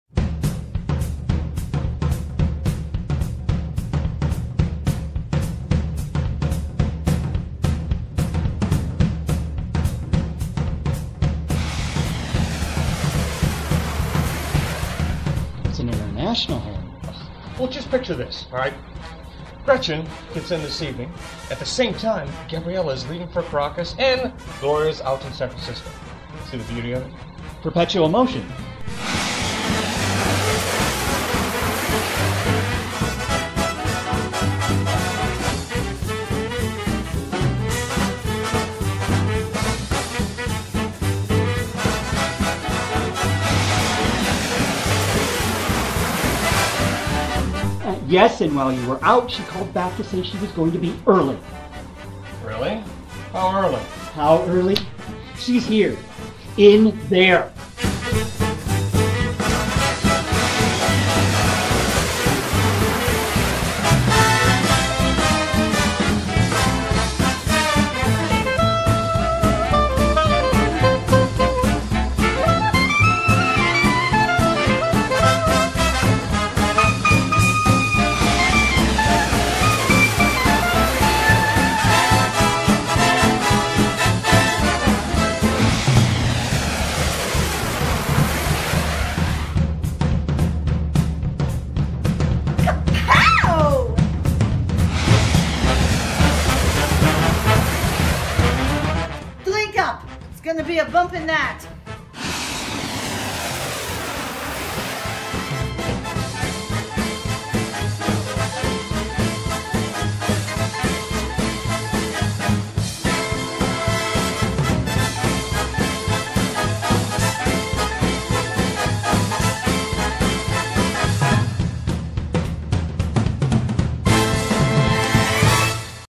Boeing Boeing by Marc Camoletti
Bernard is an American bachelor in Paris who believes in "flying the friendly skies" and he couldn't be happier in life.  After all, he has a spacious flat in Paris and three gorgeous flight attendants all engaged to him each not knowing about the others.  With careful attention to flight schedules Bernard has successfully managed to keep it so that only one fiance is in town at a time.